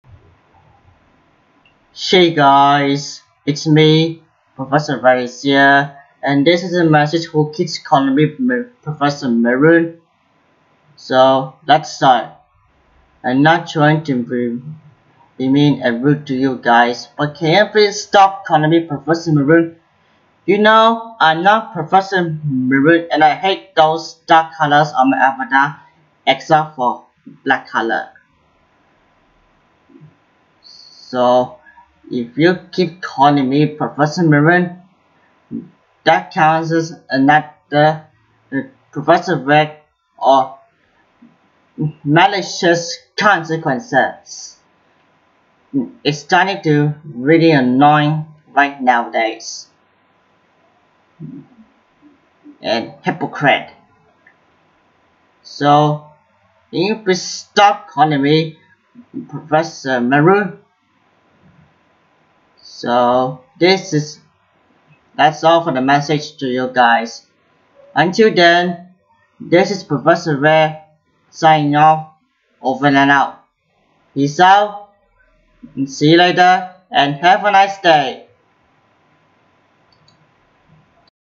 A Message To People Who Keeps Calling Me A Professor Maroon
Well, people, who someone calls me a Professor Maroon! I'm not trying to be mean or rude to you, but can you please stop calling me a Professor Maroon? You know I'm not Professor Maroon and I hate those dark colors on my avatar (except for Black)! I'm Professor Red, you know! This is getting really annoying, cringe, overused, and hypocritical right nowadays! And if you keep calling me Professor Maroon, you will get serious/Professor Red/malicious consequences! Got that? Do you hear me guys?